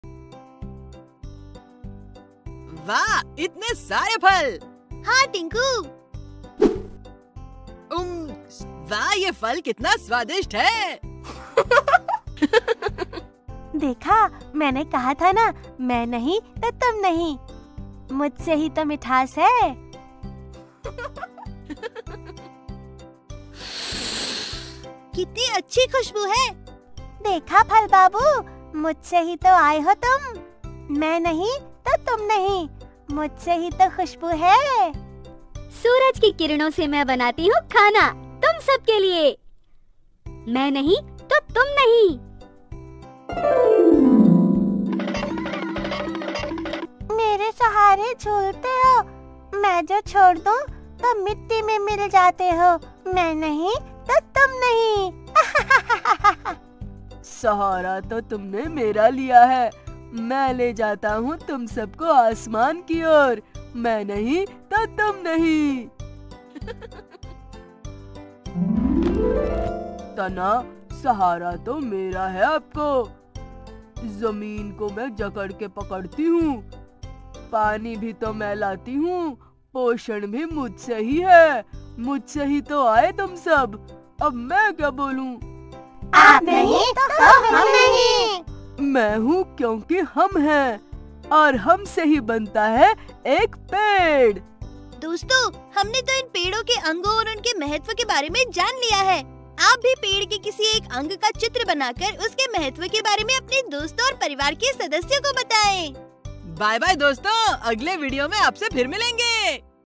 🌿पौधों के मजेदार हिस्से🌺| Parts of Plants | Kids' Video | Ages 3-8
Timestamps:
0:04- इतने सारे फल!!
0:13- किस्से है मिठास?
0:28- फूल और उनकी खुशबू
0:37: खाना कौन बनाता है?
0:57- शाखाओं की भूमिका
1:28- कौन है सबसे खास?

In this video, the different parts of plants and their main roles are highlighted. Join Tinku and Smiley as they learn about these!

इस वीडियो में पौधों के विभिन्न भागों और उनकी मुख्य भूमिकाओं पर प्रकाश डाला गया है।
 इनके बारे में जानने के लिए टिंकू और स्माइली से जुड़ें!